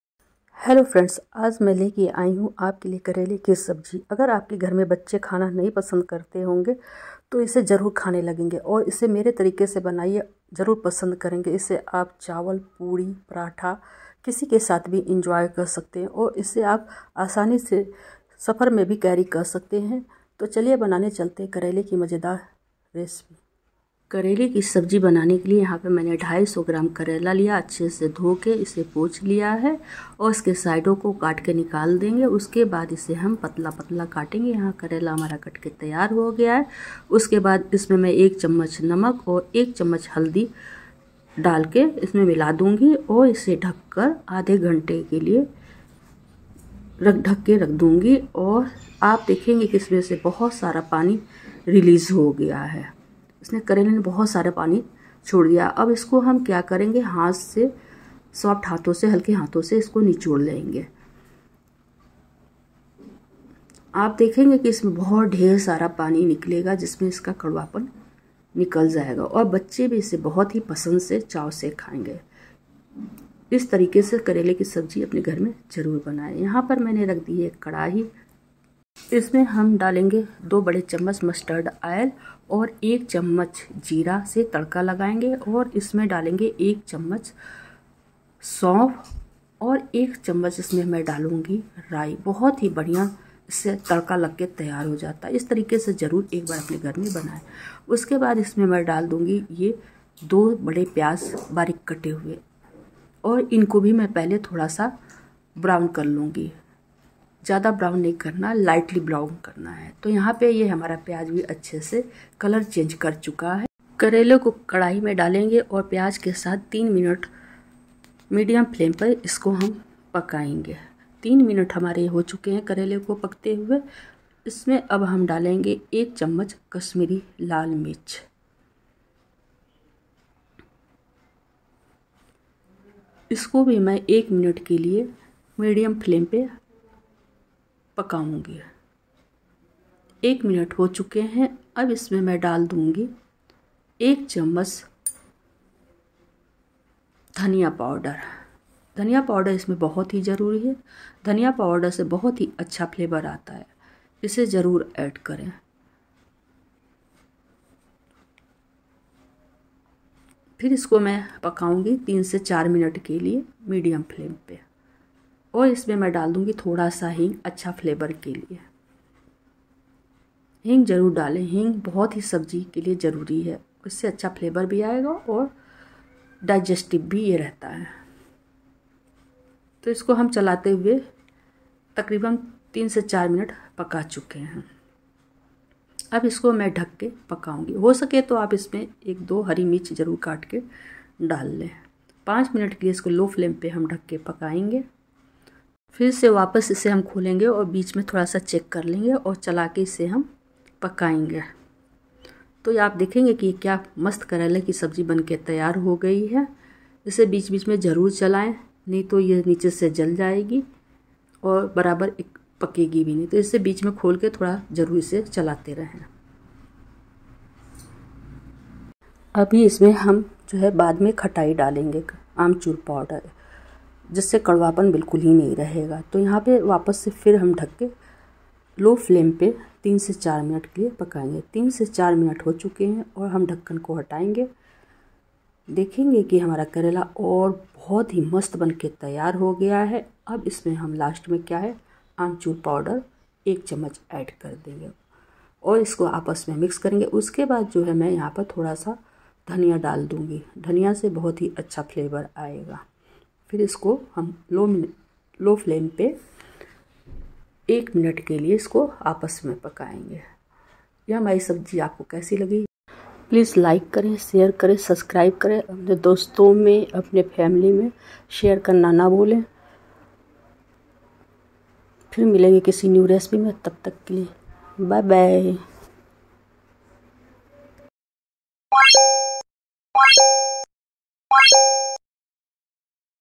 Karela Pyaz Recipell बिना कडवा लगे सब्जी कैसे बनाये । करेले कि सब्जी | Bittergourd Onion Sabzi ll
karelekisabji crispykarela करेले की मसालेदार स्वादिस्ट सब्जी फ्राई बनाने का विधि-Crispy Karela recipell How to make masala karela recipell